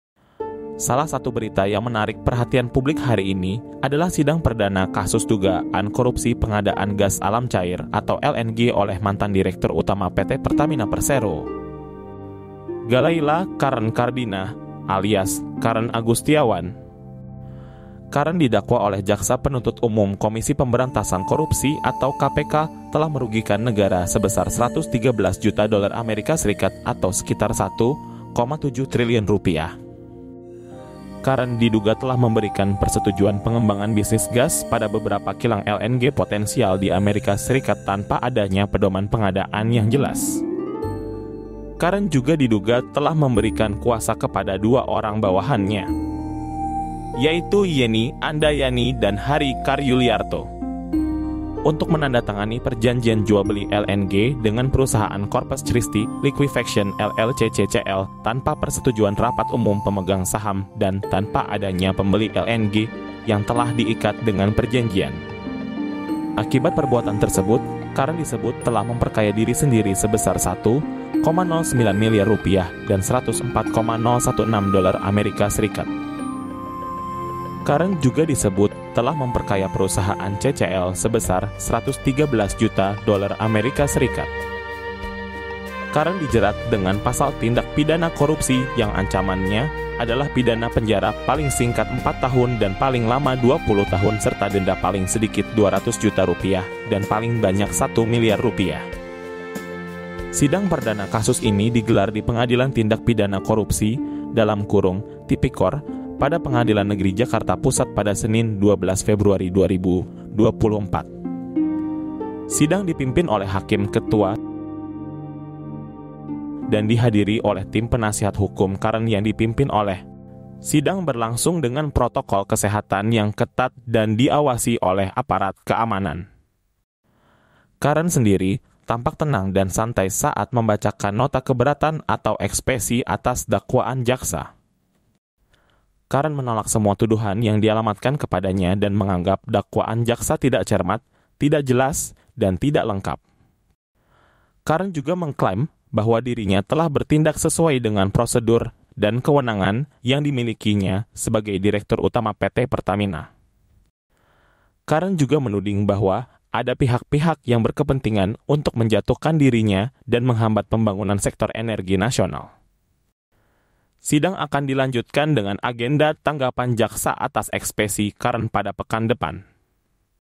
Sidang Perdana Kasus Dugaan Korupsi Mantan Direktur Pertamina
Mantan Direktur Utama Pertamina Karen Agustiawan usai menjalani sidang dakwaan di Pengadilan Tipikor, Jakarta, Senin (12/2/2024).

Karen Agustiawan didakwa merugikan negara sebesar 113 Juta US Dolar terkait kasus dugaan tindak pidana korupsi pengadaan gas alam cair atau liquified natural gas (LNG).